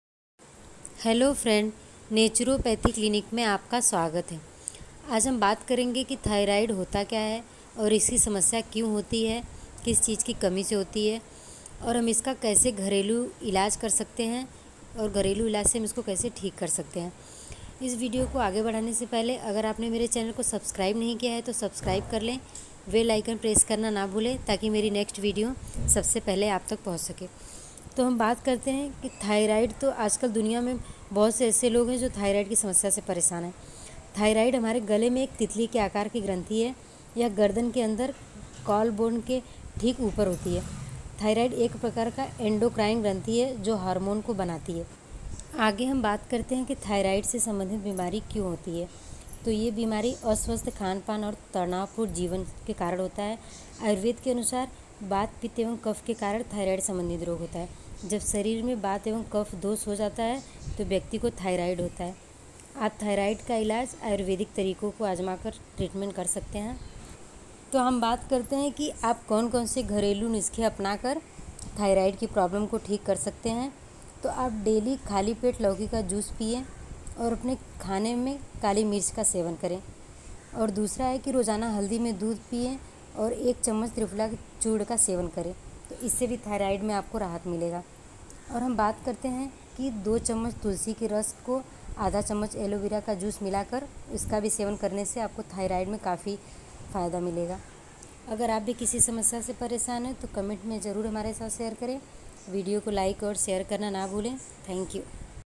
Thyroid का इलाज करे आयुर्वेदिक औषधि से। राहत मिलेगी घरेलु नुस्खे से।
लौकी के जूस का सेवन करें सुबह खालि पेट।
खाने में काली मिर्च का प्रयोग करें।